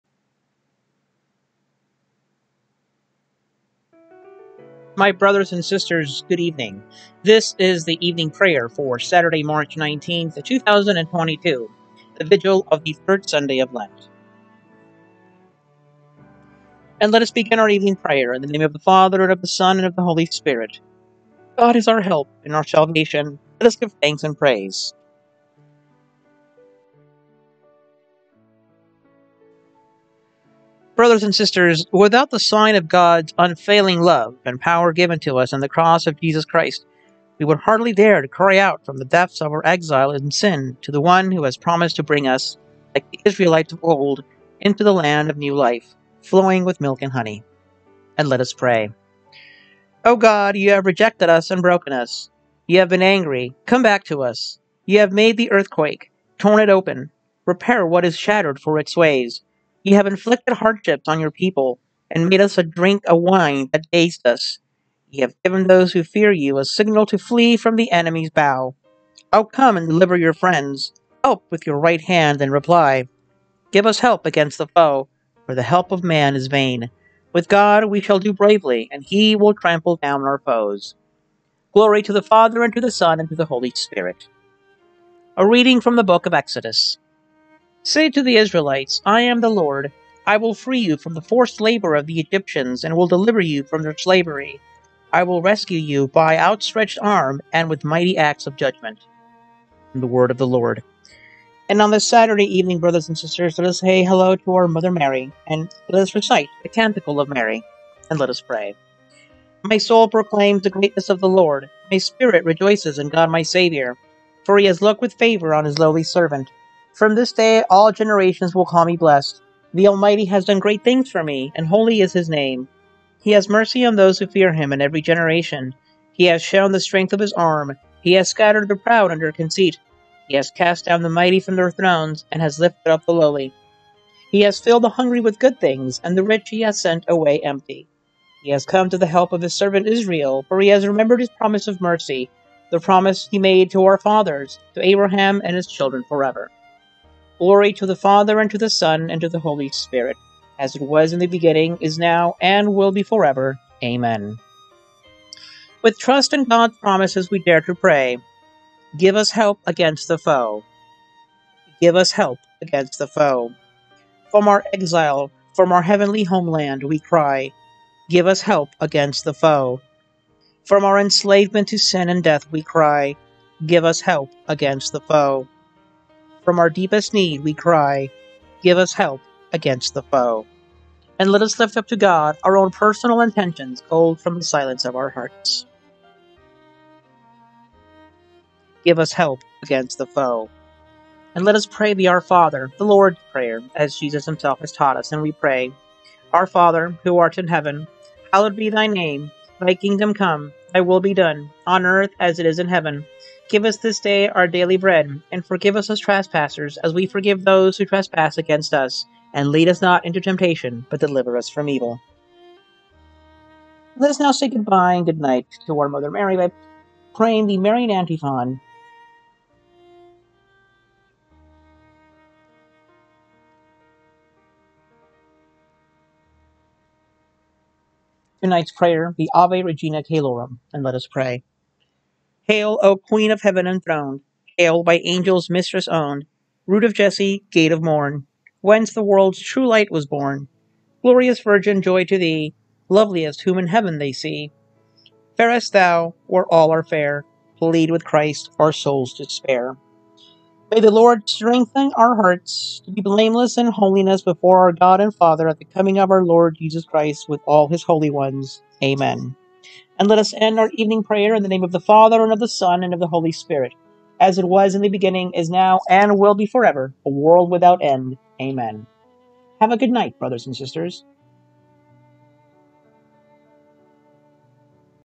Evening prayer for the Vigil of the Third Sunday of Lent
God is our help and our salvation: let us give thanks and praise! Glory to the Father. ... Amen.